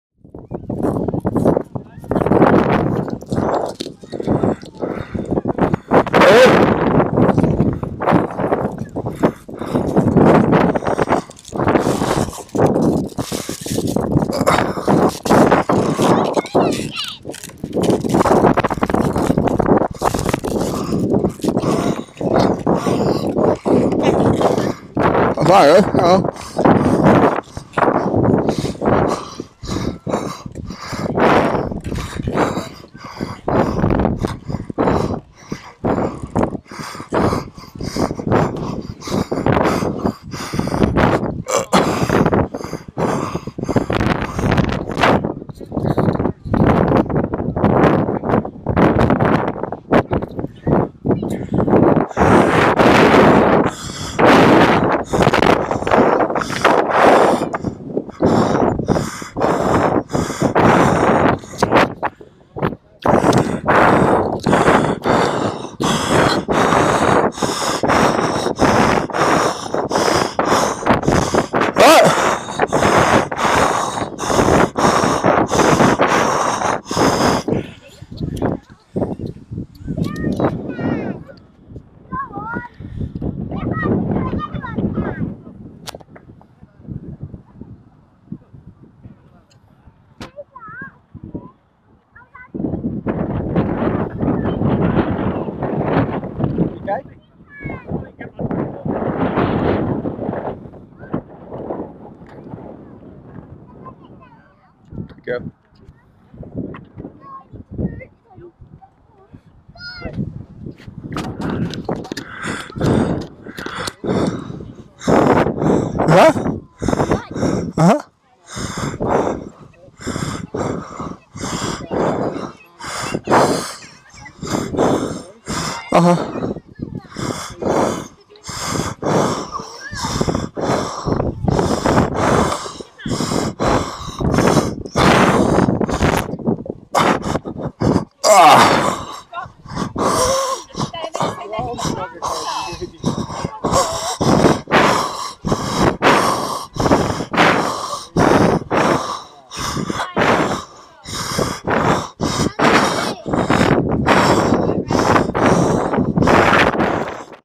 I held the GoPro with my teeth at Great Notley Country Park
I took Grace on the climbing frame at Great Notley Country park. I held the camera in my mouth. I am not as unfit as I sound.